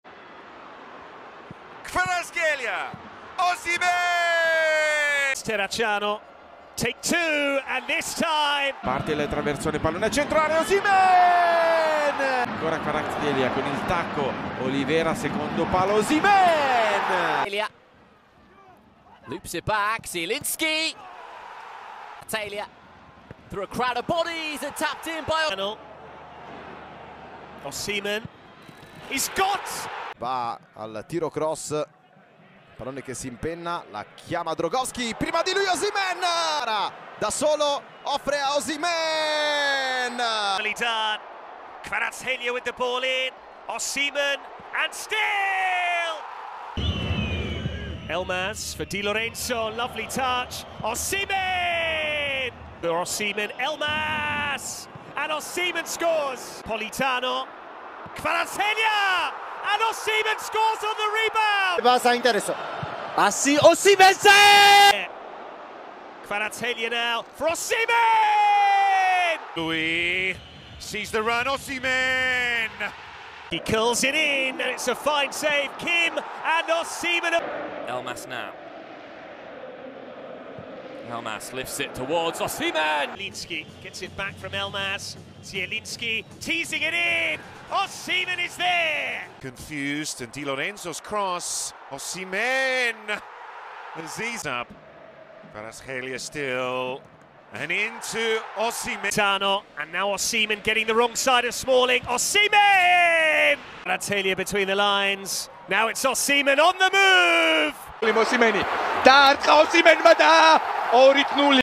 Victor Osimhen NEVER miss his chance…
Victor Osimhen never miss this goals, best goals in serie a and uefa champions league.